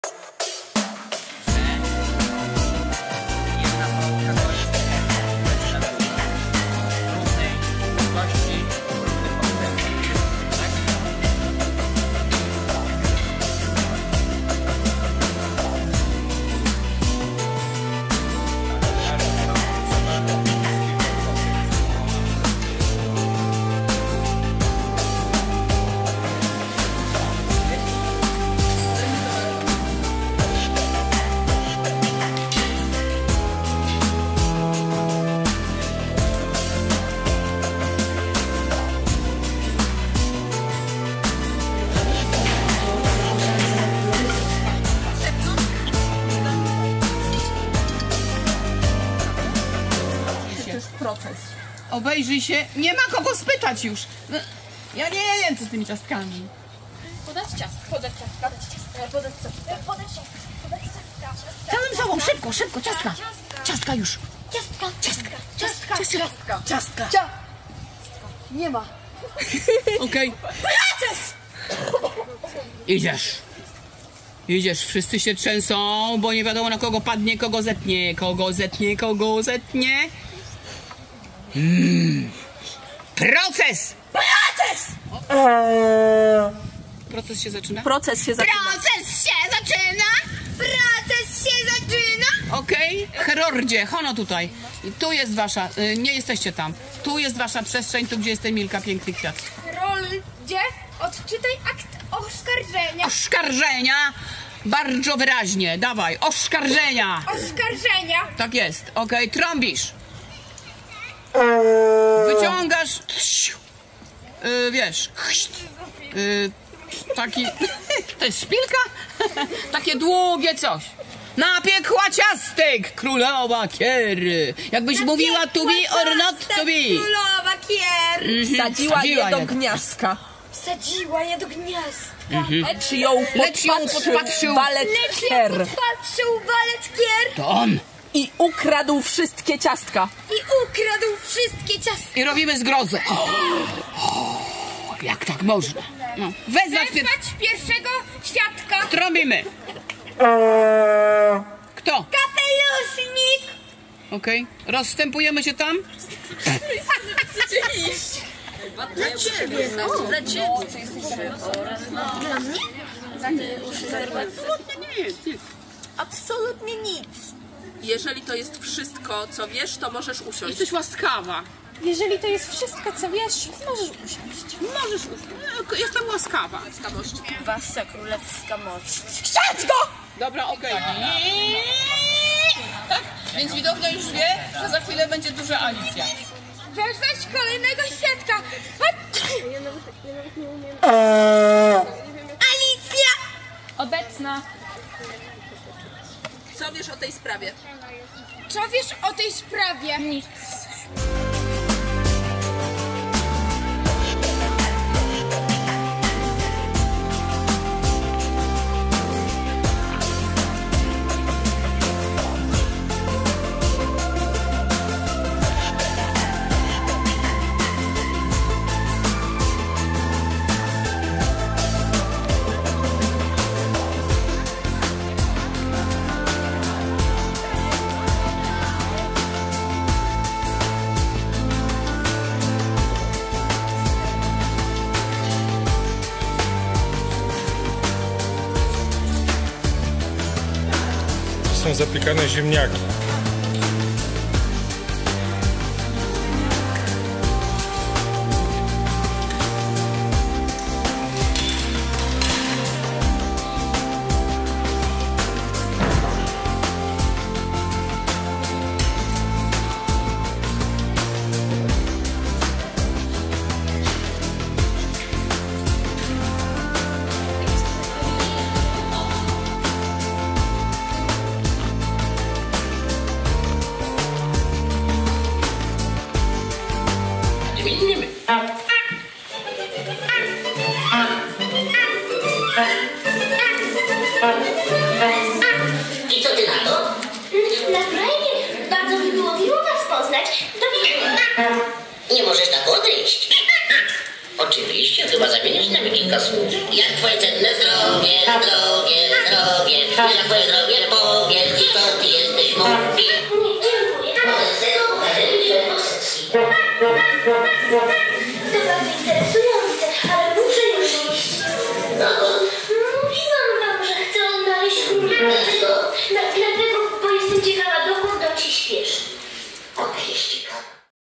Kruklanki 2024 - 03.08 - Dzień Szósty - AKT VIII - Obóz Artystyczny TOTALART
Prace nad spektaklem "Alicja" trwają pełną parą. Nie przeszkadza nam to w korzystaniu z uroków naszego pięknego Ośrodka Jakub. Dzisiaj było plażowanie a wieczorem pokaz filmu Disneya o naszej Alicji. No i co bardzo ważne, karmią nas tutaj bardzo dobrze. mamy pyszne i urozmaicone jedzonko. Dla każdego coś dobrego.